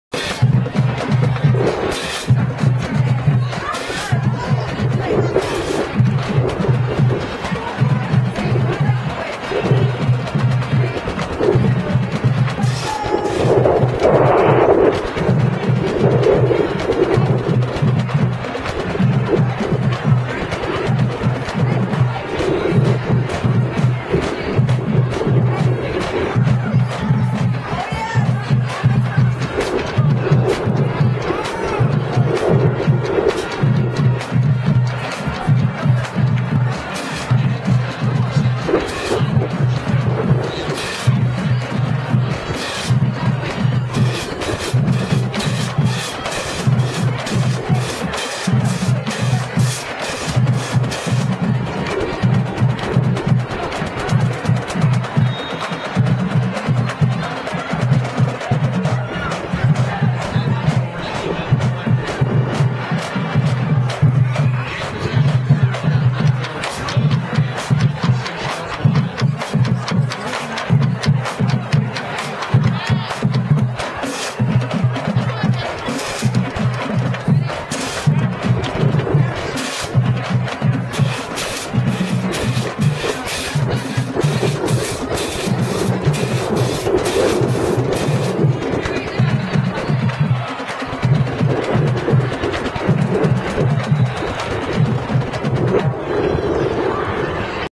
The John Jay Mustang Band performs at the football game vs Marshall (Senior Night) on November 9, 2012